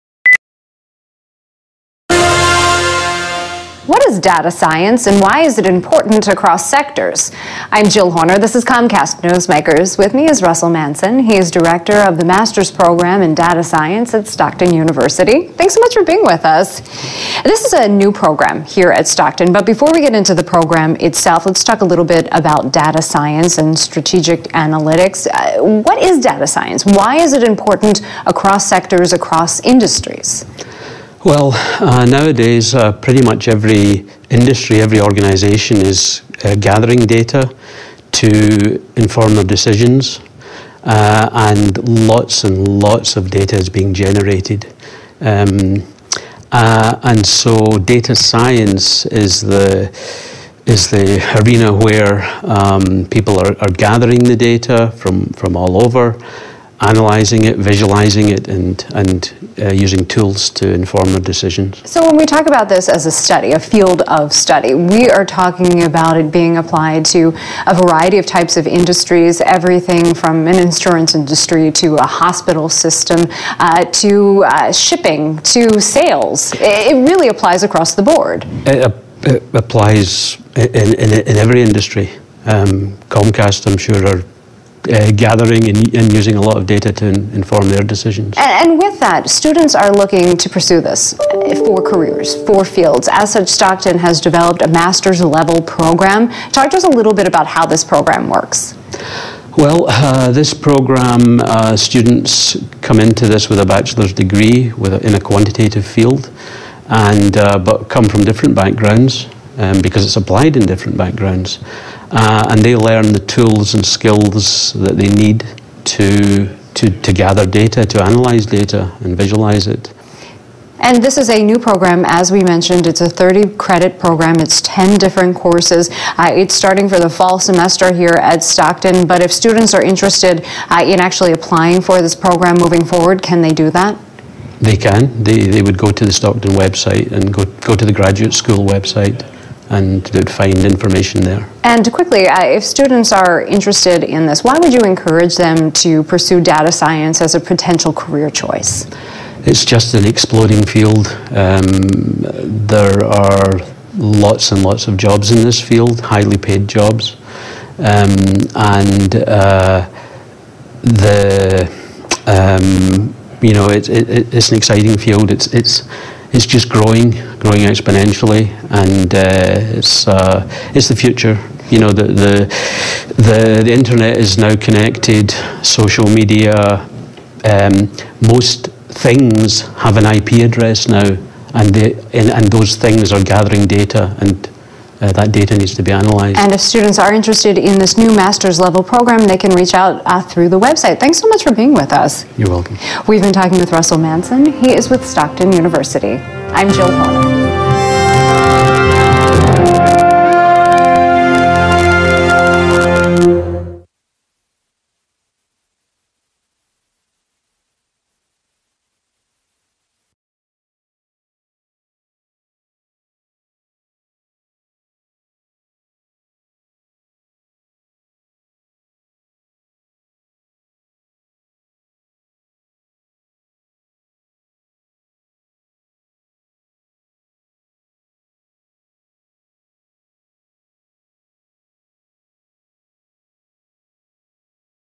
Masters Program in Data Science and Strategic Analytics
Jill Horner speaks with Russell Manson, Director of Master’s Program and Data Service from the New Master’s Program in Data Science and Strategic Analytics, about the New Master’s Program. Interview recorded 8/23/2017. For more videos and information about your community, go to ComcastNewsmakers.com.